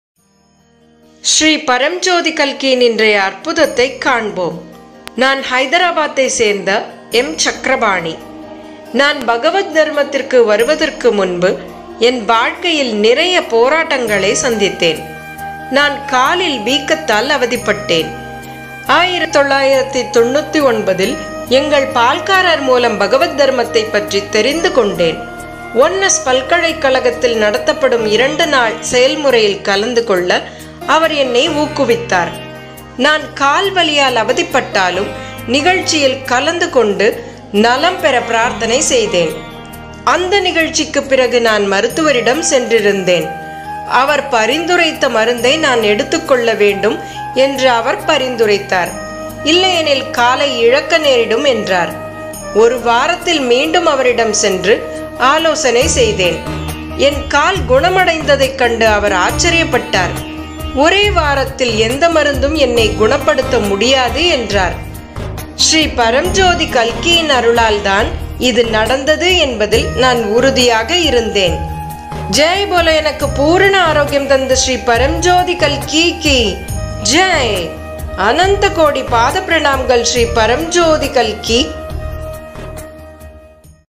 10/2.ஸ்ரீ பரம்ஜோதி கல்கியின் அருளால் மட்டுமே ஒரே வாரத்தில் குணமடைந்தேன்.
நான் காலில் வீக்கத்தால் அவதிப்பட்டேன். நான் இரண்டு நாட்கள் செயல்பாட்டில் கலந்துகொண்டு நல்ல ஆரோக்கியத்திற்காக பிரார்த்தனை செய்தேன். எனக்கு பூரண ஆரோக்கியத்தை தந்த ஜெய் போலோ ஸ்ரீ பரம்ஜோதி கல்கி கி ஜெய். அனந்த கோடி பாத பிரணாமங்கள் ஸ்ரீ பரம்ஜோதி கல்கி.